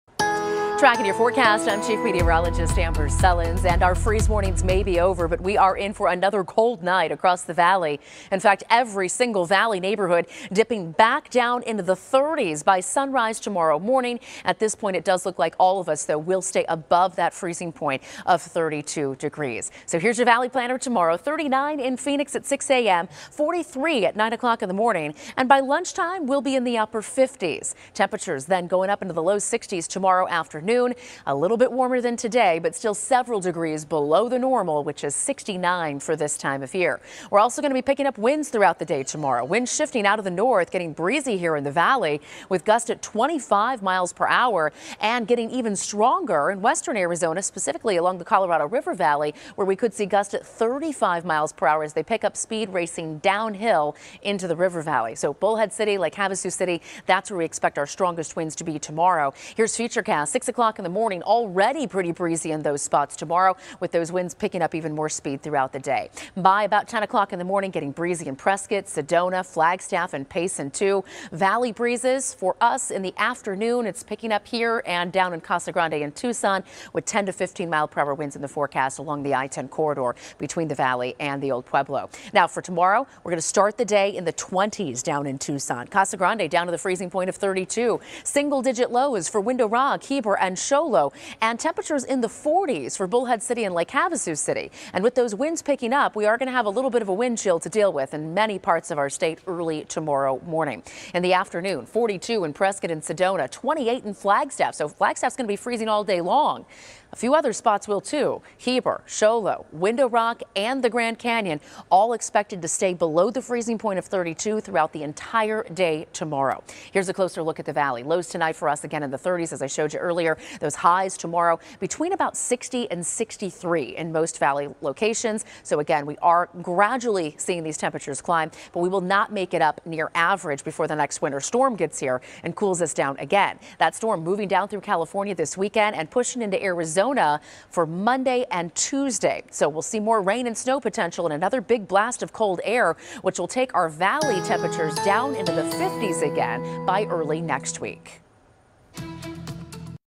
Another cold night for the Valley
The Valley will dip in the 30s overnight, warmer temperatures expected for the afternoon.